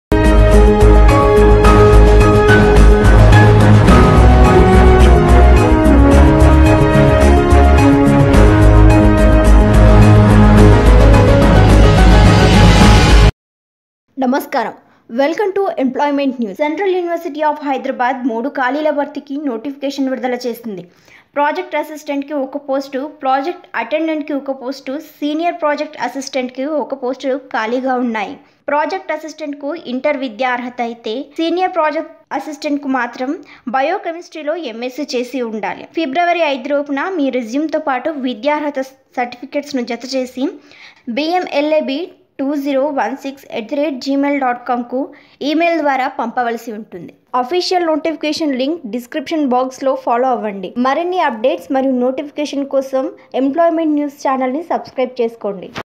Local Jobs Update 2020 | No exam Fees send your bio data latest HYD jobs | AP and TS jobs
University Of Hyderabad Project Assistant Jobs 2020 | హైదరాబాద్ యూనివర్సిటీ నుండి ఉద్యోగాల భర్తీ
హైదరాబాద్ యూనివర్సిటీ నుండి వివిధ ఉద్యోగాల భర్తీకి నోటిఫికేషన్ విడుదల కావడం జరిగింది. తెలంగాణ మరియు ఆంధ్రప్రదేశ్ లోని అర్హులైన అభ్యర్థులు అందరూ ఈ పోస్టులకు దరఖాస్తు చేసుకోవచ్చు, మరియు ఈ పోస్టులకు సెలెక్ట్ అయిన అభ్యర్థులకు హైదరాబాద్ లో పోస్టింగ్ ఇవ్వడం జరుగుతుంది. University Of Hyderabad Project Assistant Jobs 2020
పోస్టుల సంఖ్య:
అన్ని విభాగాల్లో మొత్తం 3 ఖాళీల భర్తీకి నోటిఫికేషన్ విడుదల కావడం జరిగింది

విభాగాల వారీగా ఖాళీలు:
ప్రాజెక్ట్ అసిస్టెంట్ 1
ప్రాజెక్ట్ అటెండెంట్ 1
సీనియర్ ప్రాజెక్ట్ అసిస్టెంట్ 1
అర్హతలు:
ప్రాజెక్ట్ అసిస్టెంట్/ప్రాజెక్ట్ అటెండెంట్:
ఇంటర్మీడియట్ పాస్ అయి ఉండాలి

సీనియర్ ప్రాజెక్ట్ అసిస్టెంట్:
బయో కెమిస్ట్రీ లేదా లైఫ్ సైన్స్ లో M.Sc చేసి ఉండాలి

దరఖాస్తు చేసుకునే విధానం:
ఈ పోస్టులకు దరఖాస్తు చేసుకునే అభ్యర్థులు పైన ఇవ్వబడిన తేదీలోపు క్రింద ఇవ్వబడిన ఈమెయిల్ అడ్రస్ కు తమ బయోడేటాను పంపవలసి ఉంటుంది.

ఈ-మెయిల్ అడ్రస్:
మరిన్ని వివరాలు తెలుసుకోవడం కొరకు క్రింద ఇవ్వబడిన వెబ్ సైట్ ను సంప్రదించగలరు.